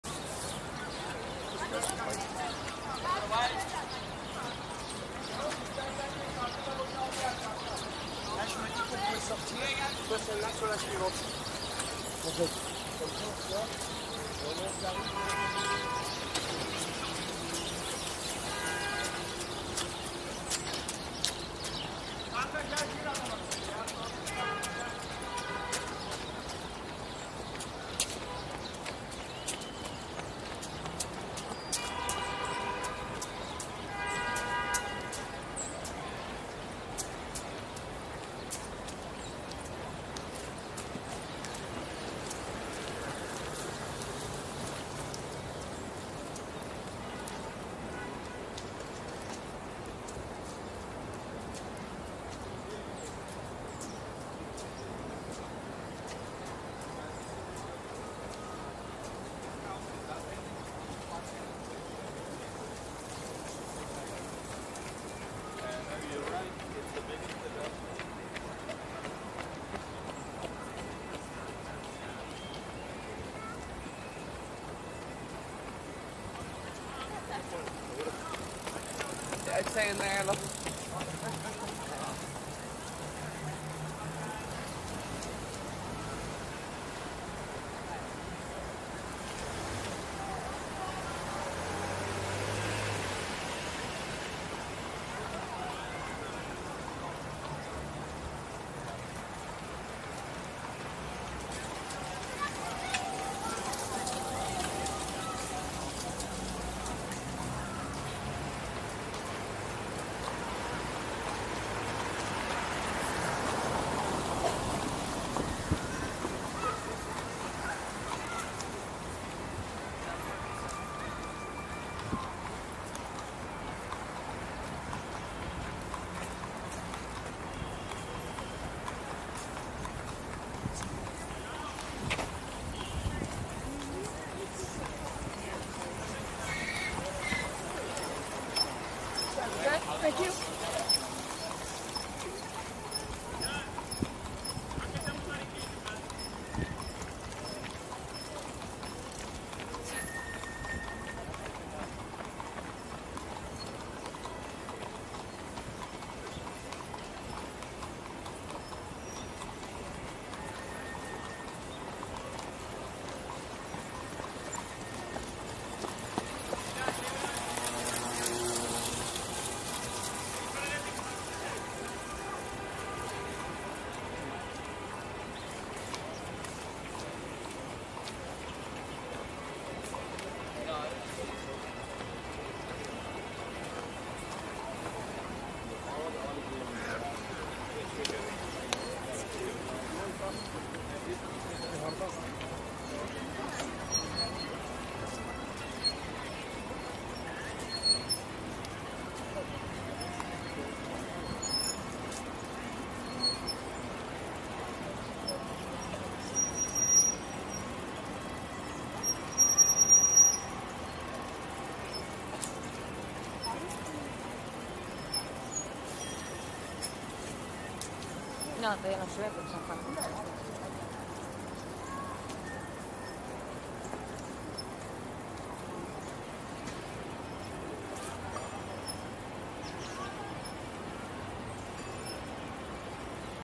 New York City - Central Park 1
Here's a 360° immersive video from New York City. It's in Central Park. I used the original Insta 360 camera. It was a little overcast and started to rain right after I was done.